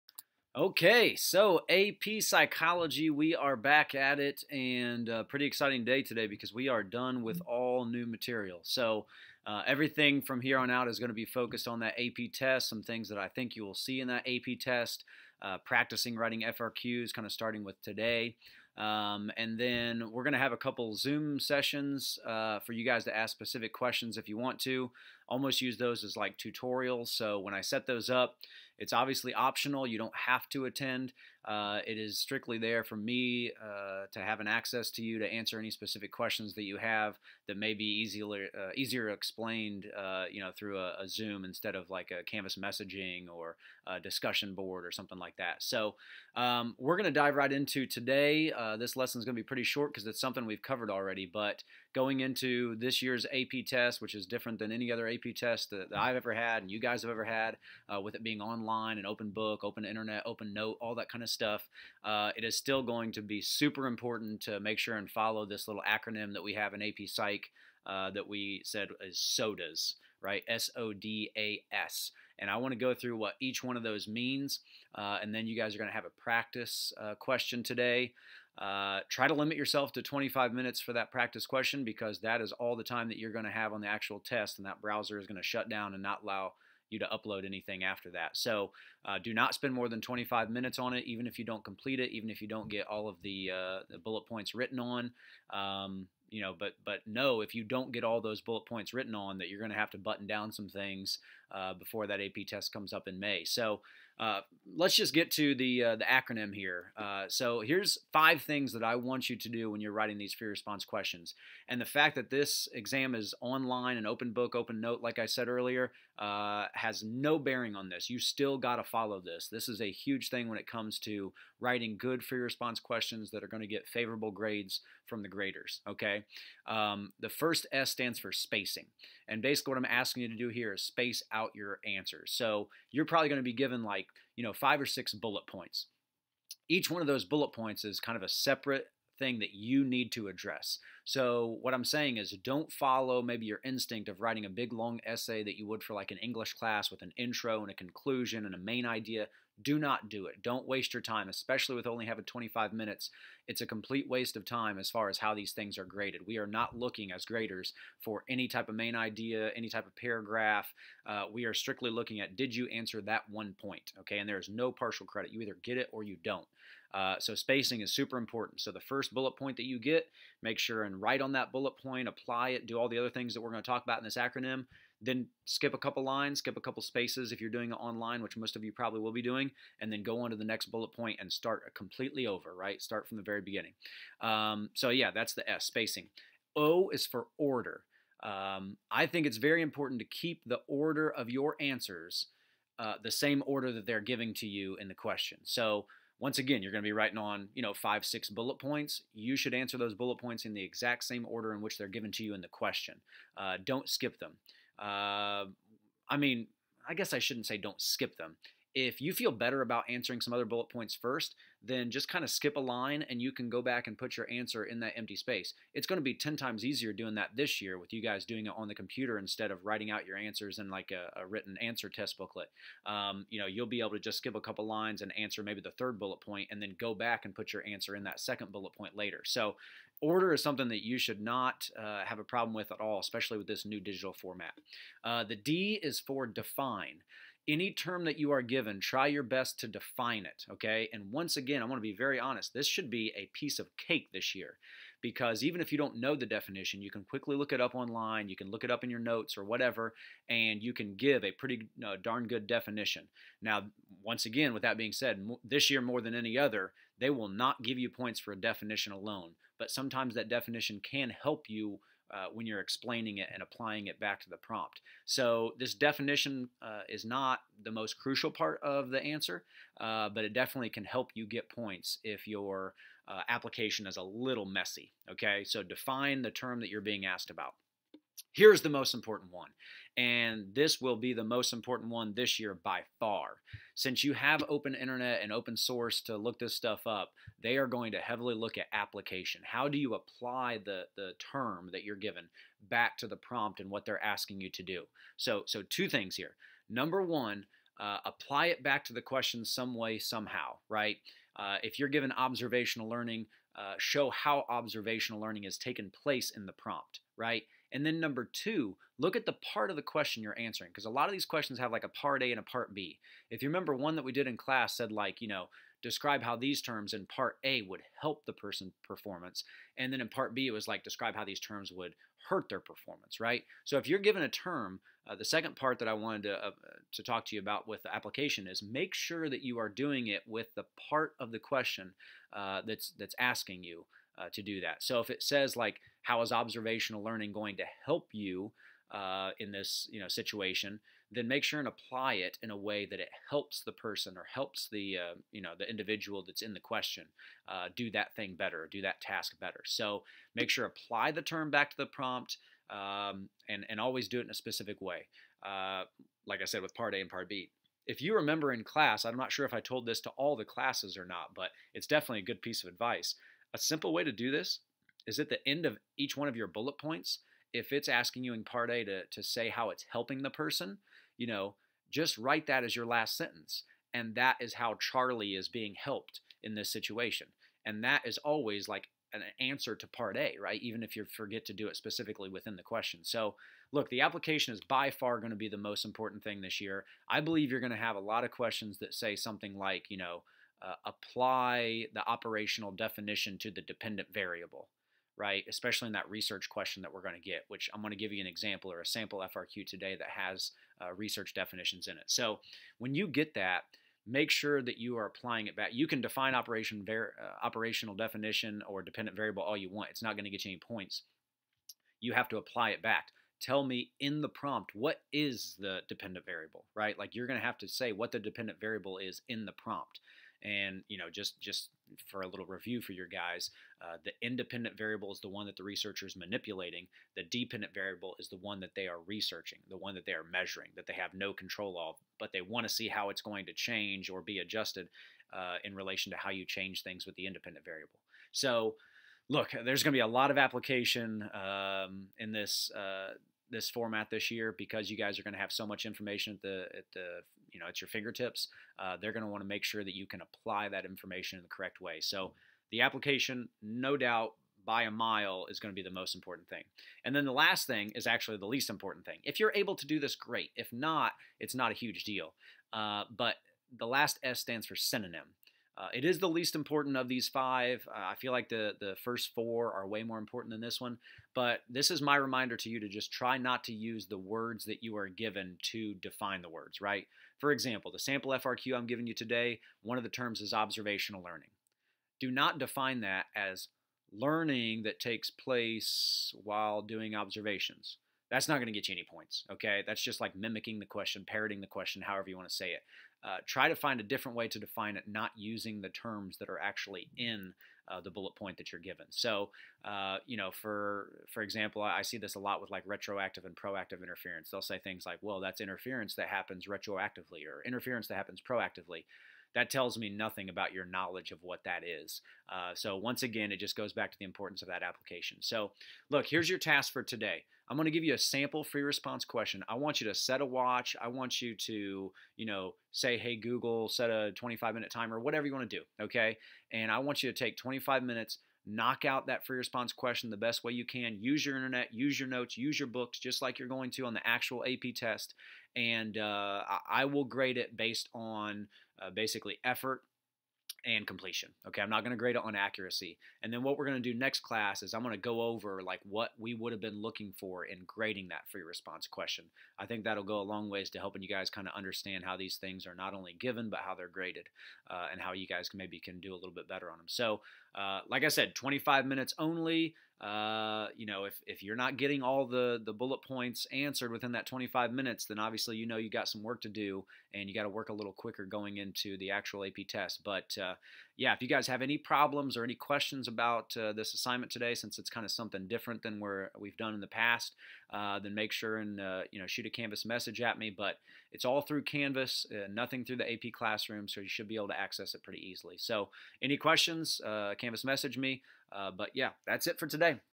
How to write AP Psychology Free Response Questions
Quick video on how to write Free Response Questions for the AP Psychology 2020 Exam. Made by Jared Turner, teacher at Yorktown High School, and College Board Reader for the AP Psychology Exam